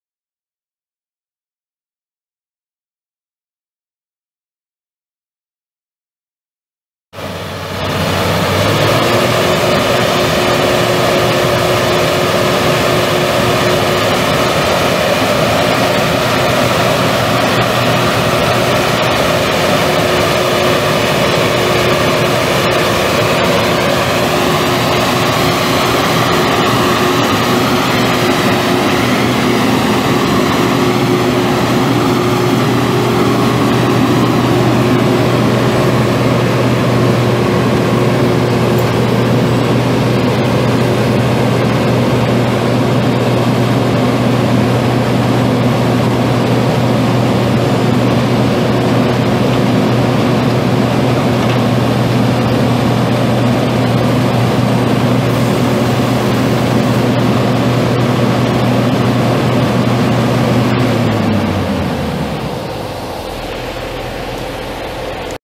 2006 Ag-Chem RoGator 874 self-propelled sprayer for sale | no-reserve auction November 15, 2017
2006 Ag-Chem RoGator 874 self-propelled sprayer
4,009 hours on meter
Caterpillar 3126 six cylinder turbo diesel engine
Hydrostatic transmission
800 gallon stainless steel solution tank
90'W boom
Five boom sections
Hydraulic boom fold
5-way nozzles
20" nozzle spacing
Chemical eductor
Rinse tank
Foam marker tank
Suspension seat
Work lights
Trimble EZ-Guide GPS monitor
Hydraulic ladder
Fenders
Hydraulic axle width adjust
380/90R46 row crop tires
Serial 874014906
View repairs
It's located in Glasgow, MO, and we're selling it to the highest bidder regardless of price.